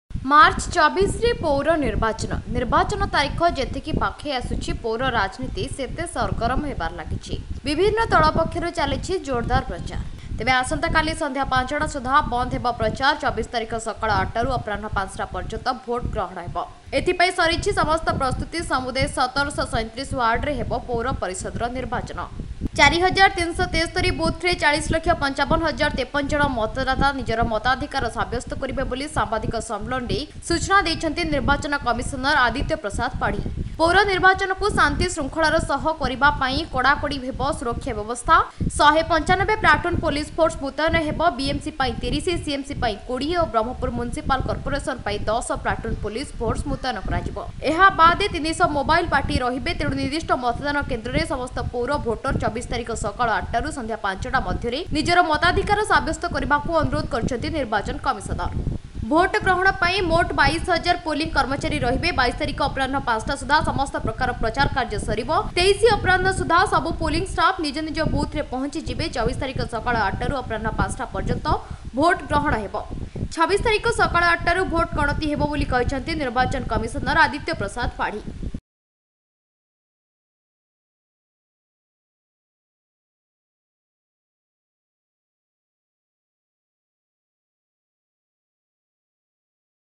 State Election Commissioner Aditya Prasad Padhi Speaks On Odisha ULB Polls
ମାର୍ଚ୍ଚ ୨୪ରେ ପୌର ନିର୍ବାଚନ | ନିର୍ବାଚନ ତାରିଖ ଯେତିକି ପାଖେଇ ଆସୁଛି ପୌର ରାଜନୀତି ସେତେ ସରଗରମ ହେବାରେ ଲାଗିଛି | ବିଭିର୍ନ ଦଳ ପକ୍ଷରୁ ଚାଲିଛି ଜୋରଦାର ପ୍ରଚାର | ତେବେ  ଆସନ୍ତାକାଲି ସନ୍ଧ୍ୟା ୫ଟା ସୁଦ୍ଧା ବନ୍ଦ ହେବ ପ୍ରଚାର । ୨୪ ତାରିଖ ସକାଳ ୮ଟାରୁ ଅପରାହ୍ନ ୫ଟା ପର୍ଯ୍ୟନ୍ତ ହେବ ଭୋଟ ଗ୍ରହଣ । ଏଥିପାଇଁ ସରିଛି ସମସ୍ତ ପ୍ରସ୍ତୁତି | ସମୁଦାୟ ୧୭୩୭ ୱାର୍ଡରେ ହେବ ପୌର ପରିଷଦ ନିର୍ବାଚନ । ୪୩୭୩ବୁଥରେ ୪୦ଲକ୍ଷ ୫୫ହଜାର ୫୩ଜଣ ମତଦାତା ନିଜର ମତାଧିକାର ସାବ୍ୟସ୍ତ କରିବେ ବୋଲି  ସାମ୍ବାଦିକ ସମ୍ମିଳନୀରେ ସୂଚନା ଦେଇଛନ୍ତି ନିର୍ବାଚନ କମିଶନର ଆଦିତ୍ୟ ପ୍ରସାଦ ପାଢୀ ।

ପୌର ନିର୍ବାଚନକୁ ଶାନ୍ତି ଶୃଙ୍ଖଳାର ସହ କରିବା ପାଇଁ କଡାକଡି ହେବ ସୁରକ୍ଷା ବ୍ୟବସ୍ଥା । ୧୯୫ ପ୍ଲାଟୁନ ପୋଲିସ ଫୋର୍ସ ମୁତୟନ ହେବେ । ବିଏମିସି ପାଇଁ ୩୦, ସିଏମସି ପାଇଁ ୨୦ ଓ ବ୍ରହ୍ମପୁର ମୁନିସପାଲ କର୍ପୋରେସନ ପାଇଁ ୧୦ ପ୍ଲାଟୁନ ପୋଲସି ଫୋର୍ସ ମୁତୟନ କରାଯିବ । ଏହାବାଦ ୩୦୦ ମୋବାଇଲ ପାର୍ଟି ରହିବେ। ତେଣୁ ନିର୍ଦ୍ଦିଷ୍ଟ ମତଦାନ କେନ୍ଦ୍ରରେ  ସମସ୍ତ ପୌର ଭୋଟର  ୨୪ ତାରିଖ ସକାଳ ୮ଟାରୁ ସନ୍ଧ୍ୟା ୫ଟା ମଧ୍ୟରେ ନିଜ ମତାଧିକାର ସାବ୍ୟସ୍ତ କରିବାକୁ  ଅନୁରୋଧ କରିଛନ୍ତି ନିର୍ବାଚନ କମିଶନର।

ଭୋଟ ଗ୍ରହଣ ପାଇଁ ମୋଟ ୨୨ ହଜାର ପୋଲିଂ କର୍ମଚାରୀ ରହିବେ । ୨୨ ତାରିଖ ଅପରାହ୍ନ ୫ଟା ସୁଦ୍ଧା ସମସ୍ତ ପ୍ରକାର ପ୍ରଚାର କାର୍ଯ୍ୟ ସରିବ ।  ୨୩ ଅପରାହ୍ନ ସୁଦ୍ଧା ସବୁ ପୋଲିଂ ଷ୍ଟାଫ୍ ନିଜ ନିଜ ବୁଥ୍‌ରେ ପହଞ୍ଚିଯିବେ। ୨୪ ତାରିଖ ସକାଳ ୮ଟାରୁ ଅପରାହ୍ନ ୫ଟା ପର୍ଯ୍ୟନ୍ତ ଭୋଟ ଗ୍ରହଣ ହେବ । ୨୬ ତାରିଖ ସକାଳ ୮ଟାରୁ  ଭୋଟ ଗଣତି ହେବ ବୋଲି କହିଛନ୍ତି ନିର୍ବାଚନ କମିଶନର ଆଦିତ୍ୟ ପ୍ରସାଦ ପାଢୀ ।